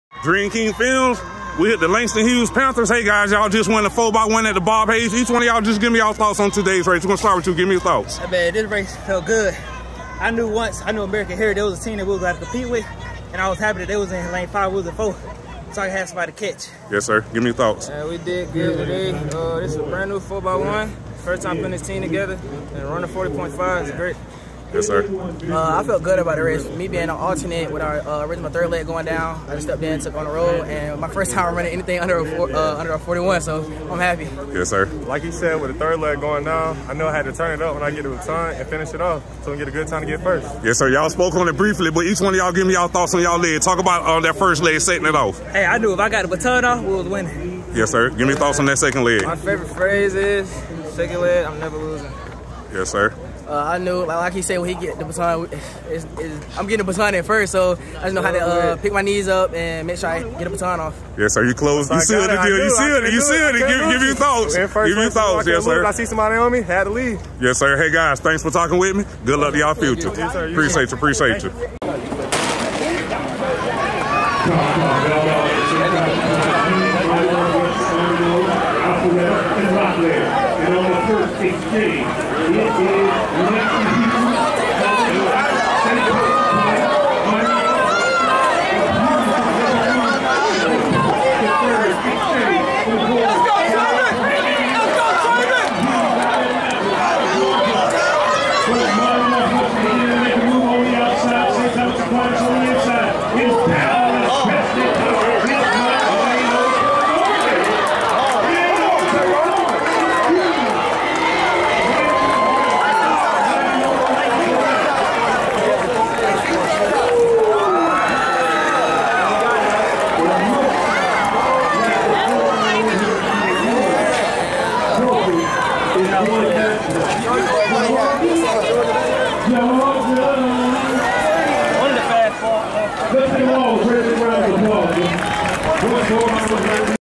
Langston Hughes Panthers 4x100 team post race interview at Bob Hayes
Langston Hughes Panthers (Fairburn,GA) 4x1 team post race interview after winning Bob Hayes Invitational with a time of 40.59